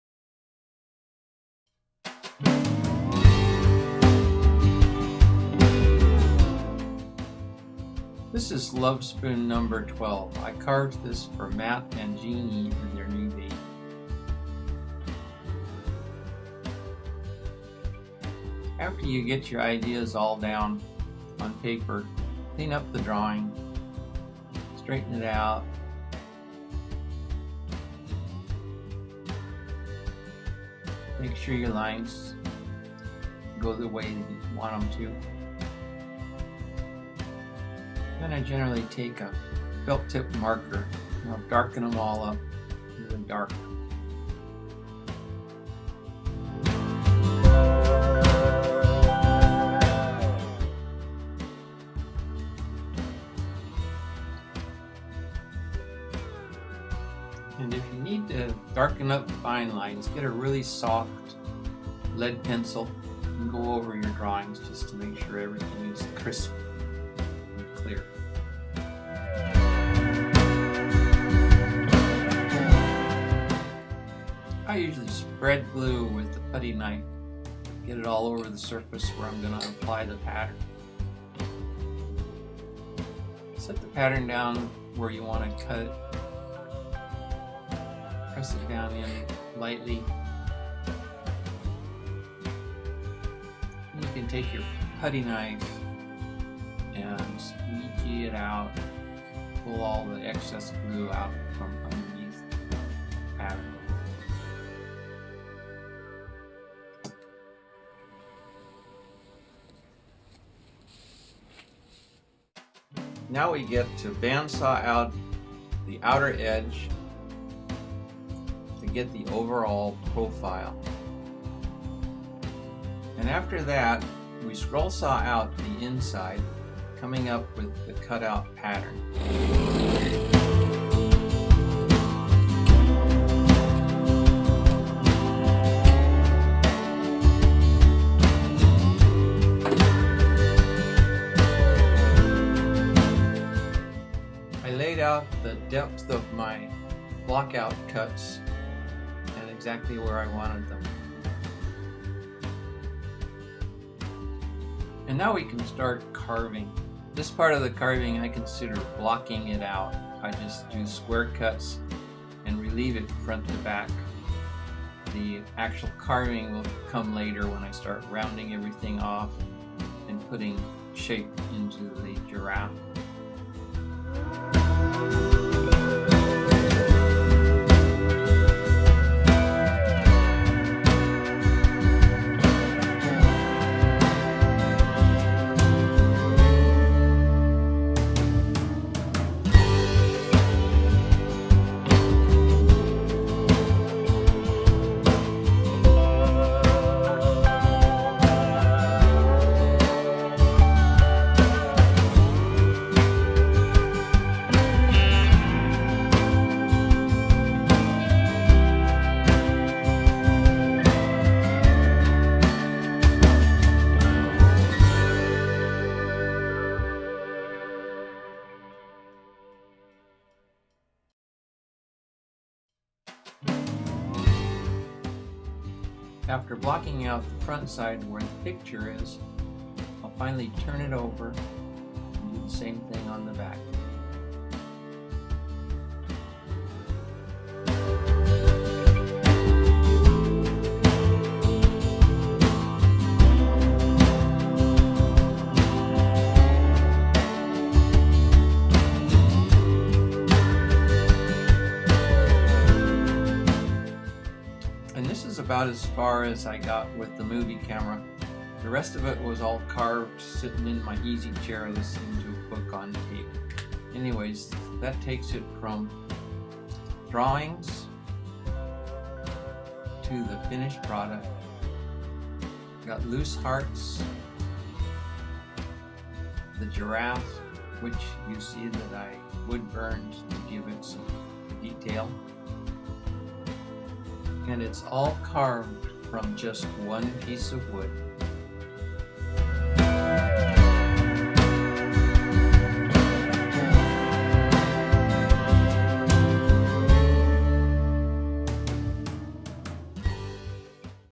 Baby Giraffe wood love spoon carving by Darryl Easter
Baby Giraffe love spoon carved by Darryl Easter